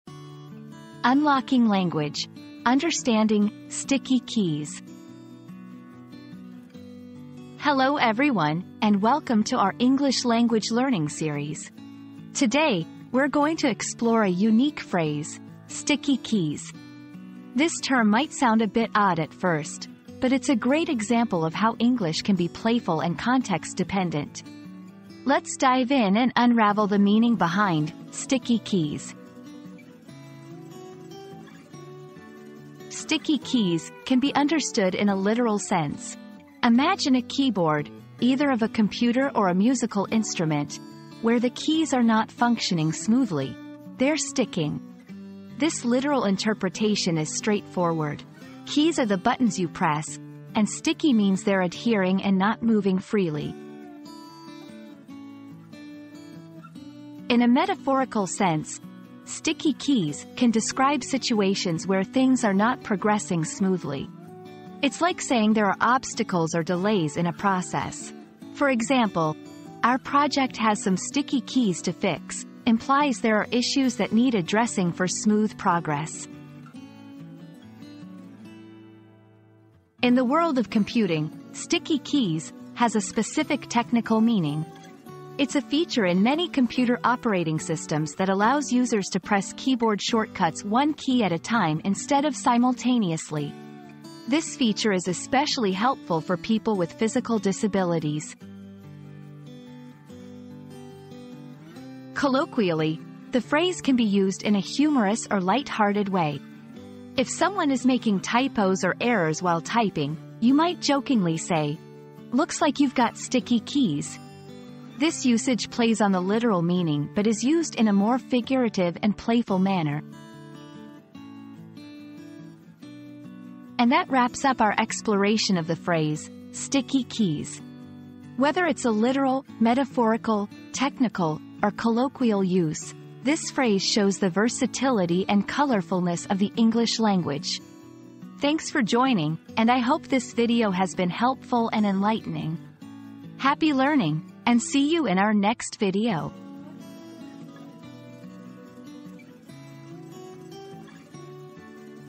Unlocking Language: Understanding "Sticky Keys"
00:00 • Introduction - Unlocking Language: Understanding "Sticky Keys"
00:32 • Literal Meaning
00:56 • Metaphorical Usage
01:19 • Technical Meaning
01:41 • Colloquial Use